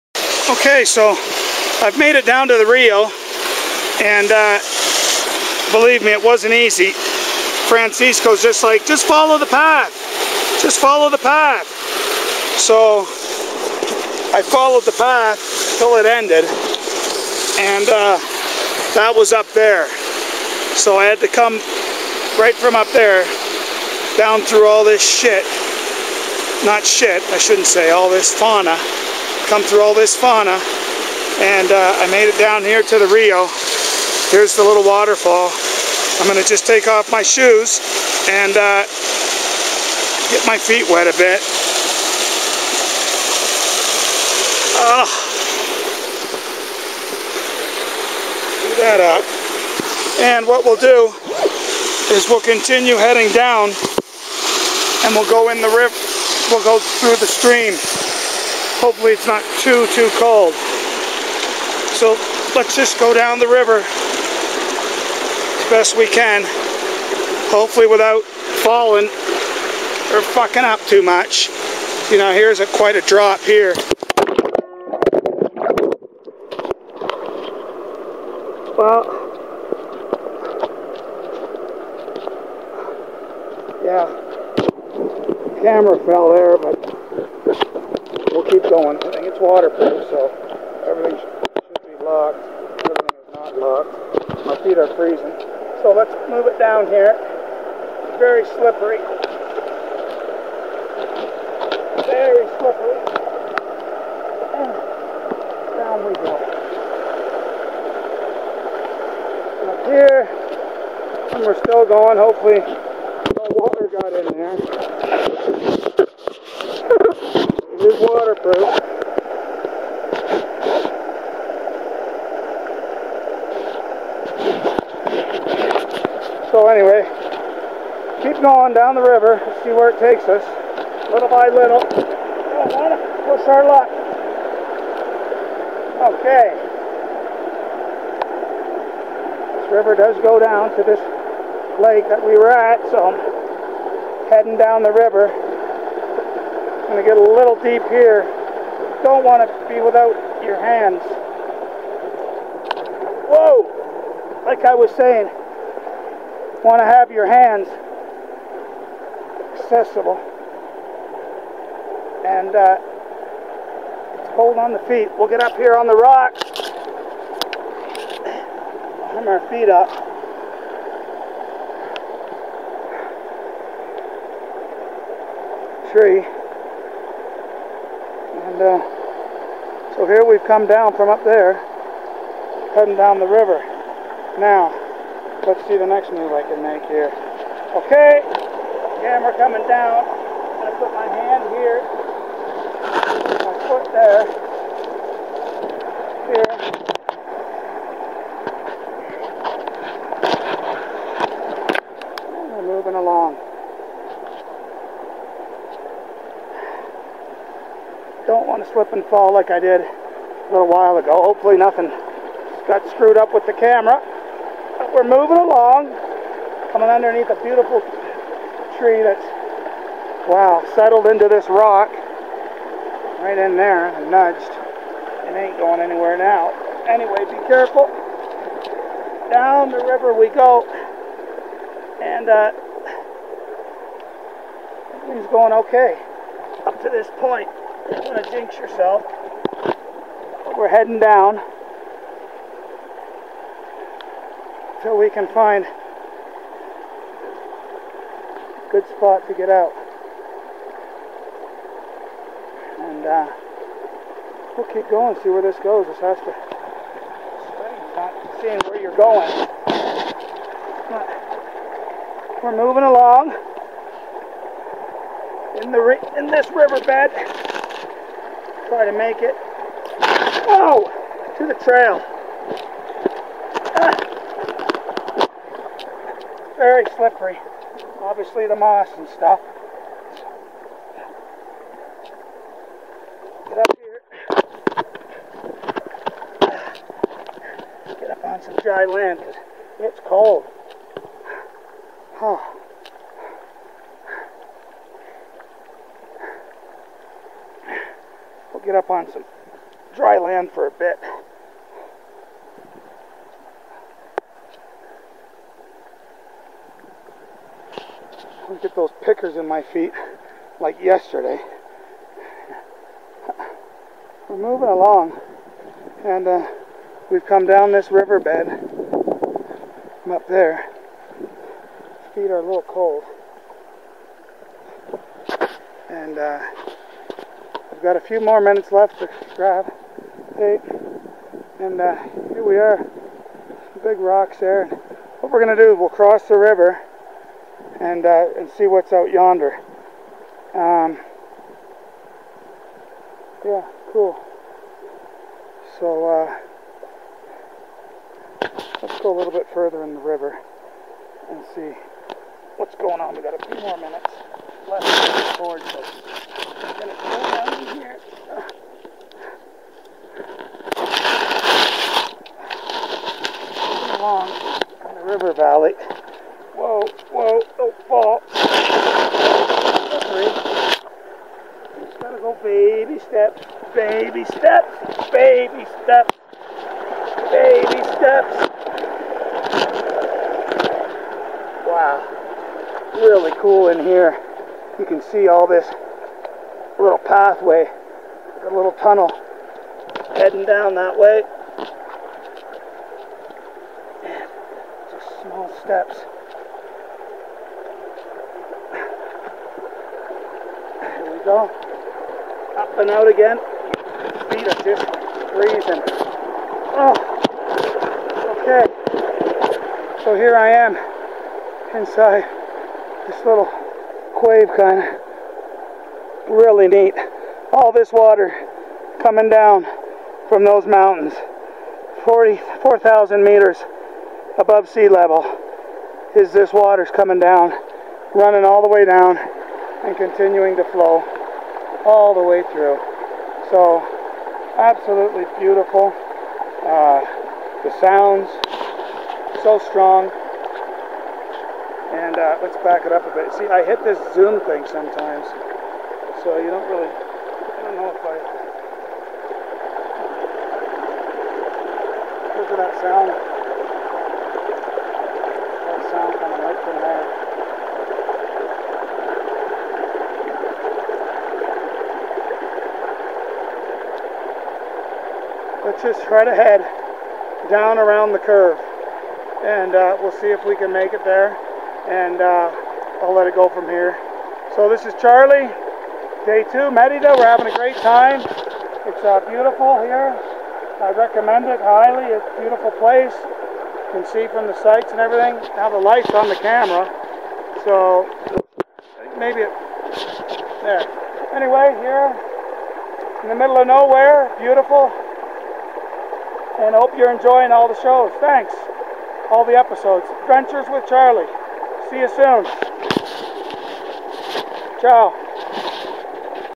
LOST IN THE ANDEA MOUNTAINS, MERIDA, VENEZUELA
HEY A FUNNY VIDEO OF HAVING TO FOLLOW A STREAM 4000 M ABOVE SEA LEVEL (ICE WATER) TO FIND MY WAY BACK TO THE PATH. GREAT TIMES.ENJOY